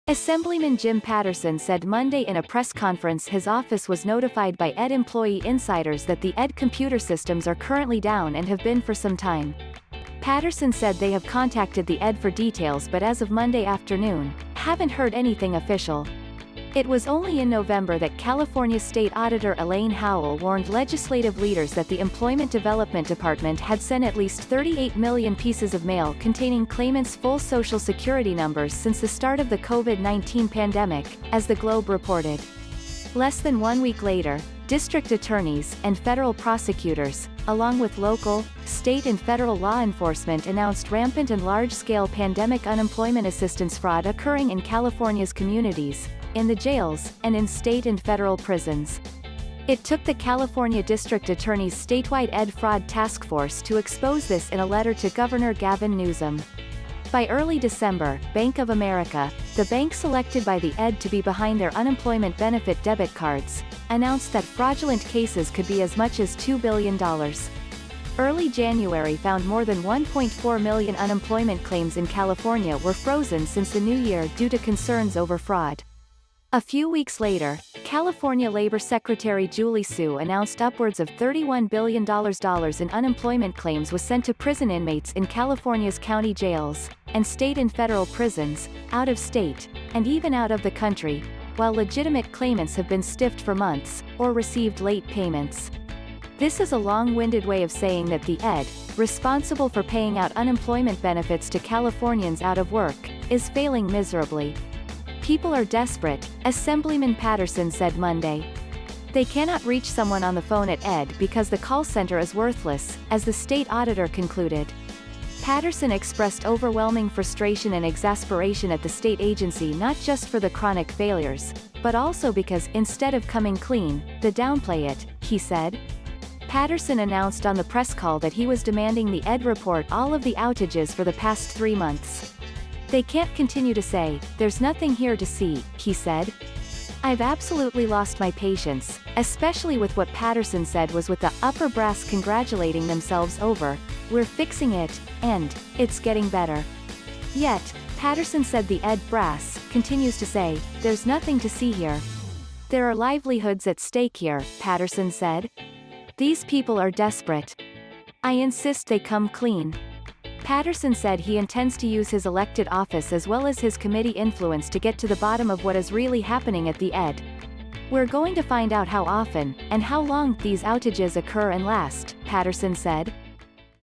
EDD Computers GO Down Again Following Numerous Breakdowns and Fraud
Assemblyman Jim Patterson said Monday in a press conference his office was notified by EDD employee insiders that the EDD computer systems are currently down and have been for some time. Patterson said they have contacted the EDD for details but as of Monday afternoon, haven’t heard anything official. It was only in November that California State Auditor Elaine Howle warned legislative leaders that the Employment Development Department had sent at least 38 million pieces of mail containing claimants’ full Social Security numbers since the start of the COVID-19 pandemic, as the Globe reported. Less than one week later, District Attorneys, and Federal Prosecutors, along with local, state and federal law enforcement announced rampant and large scale pandemic unemployment assistance fraud occurring in California’s communities, in the jails, and in state and federal prisons. It took the California District Attorney’s Statewide EDD Fraud Task Force to expose this in a letter to Gov. Gavin Newsom. By early December, Bank of America, the bank selected by the EDD to be behind their unemployment benefit debit cards, announced that fraudulent cases could be as much as $2 billion. Early January found more than 1.4 million unemployment claims in California were frozen since the New Year due to concerns over fraud. A few weeks later, California Labor Secretary Julie Su announced upwards of $31 billion dollars in unemployment claims was sent to prison inmates in California’s county jails, and state and federal prisons, out of state, and even out of the country, while legitimate claimants have been stiffed for months, or received late payments. This is a long-winded way of saying that the EDD, responsible for paying out unemployment benefits to Californians out of work, is failing miserably.“People are desperate,” Assemblyman Patterson said Monday. “They cannot reach someone on the phone at EDD because the call center is worthless, as the State Auditor concluded.”Patterson expressed overwhelming frustration and exasperation at the state agency not just for the chronic failures, but also because “instead of coming clean, the downplay it,” he said. Patterson announced on the press call that he was demanding the EDD report all of the outages for the past three months. “They can’t continue to say ‘there’s nothing here to see,'” he said. “I’ve absolutely lost my patience,” especially with what Patterson said was with the “upper brass congratulating themselves over ‘we’re fixing it’ and ‘it’s getting better.'”Yet, Patterson said the EDD “brass” continues to say, “There’s nothing to see here.”“There are livelihoods at stake here,” Patterson said. “These people are desperate. I insist they come clean!”Patterson said he intends to use his elected office as well as his committee influence to get to the bottom of what is really happening at the EDD. “We’re going to find out how often, and how long” these outages occur and last, Patterson said.